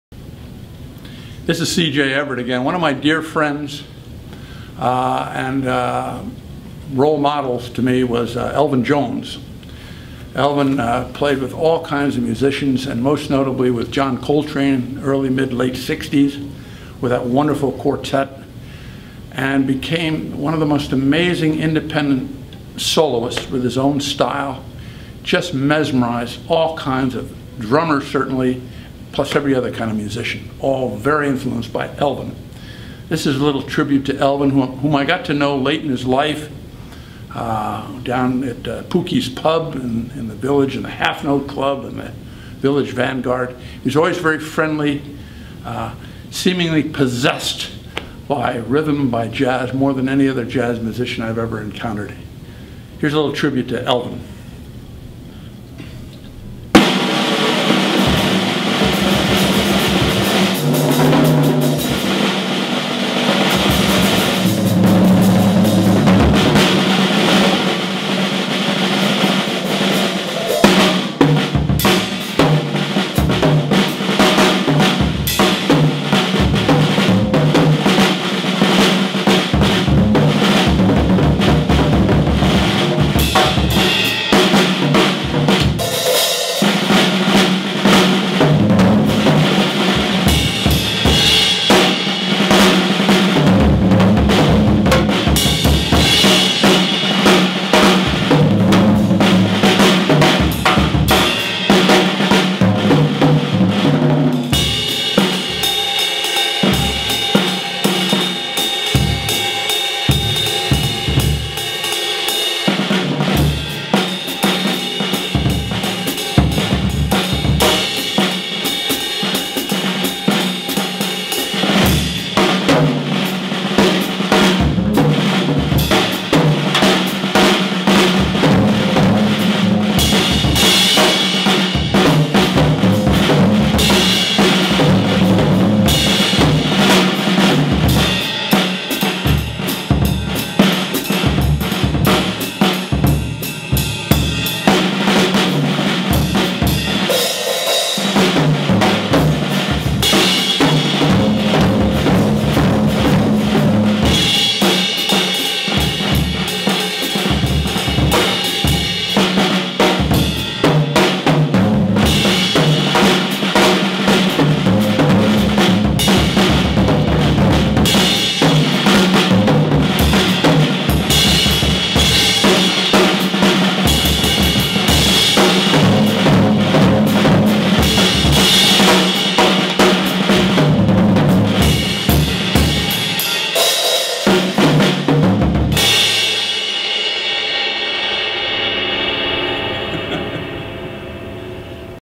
CJ Everett's Tribute to Elvin Jones
Drummer CJ Everett pays tribute to the legendary Elvin Jones